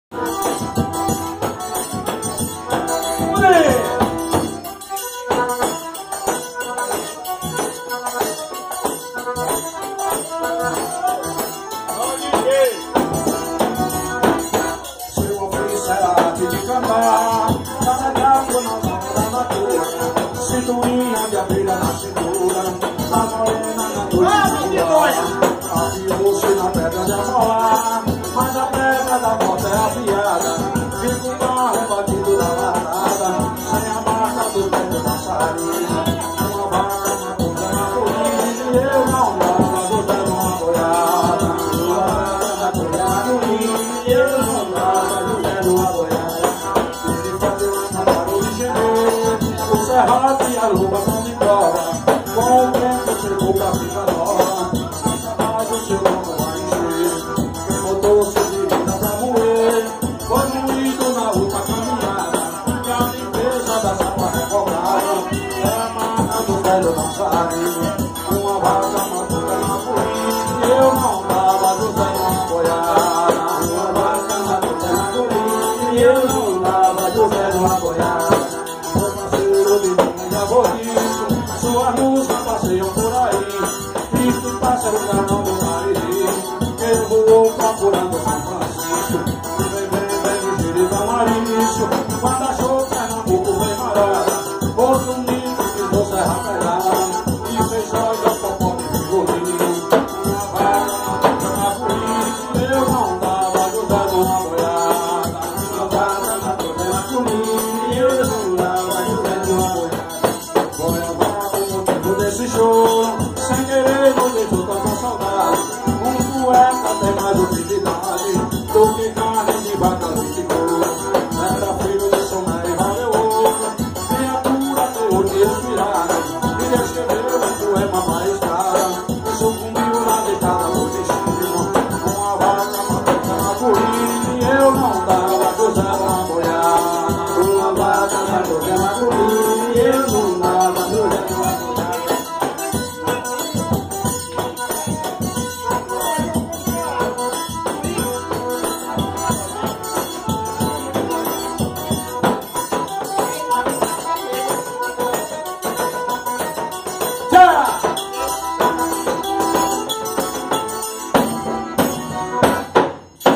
Os tropicais do forró - parte 2
Festa de aniversário de Neide Herculano 50 anos